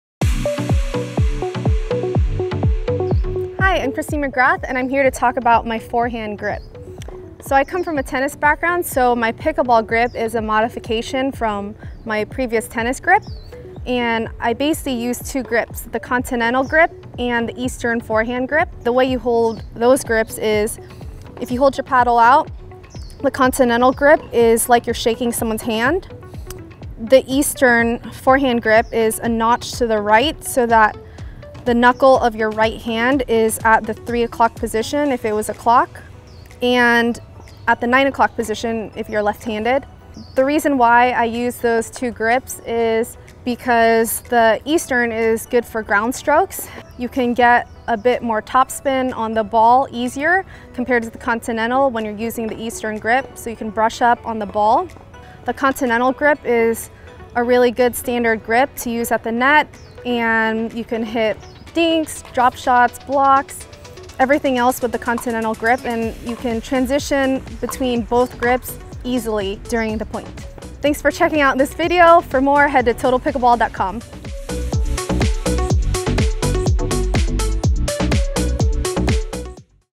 The Forehand Pickleball Grip
Learn the pickleball forehand grip with 2X US Open Champion Christine McGrath.

For more tips, and for all your gear needs head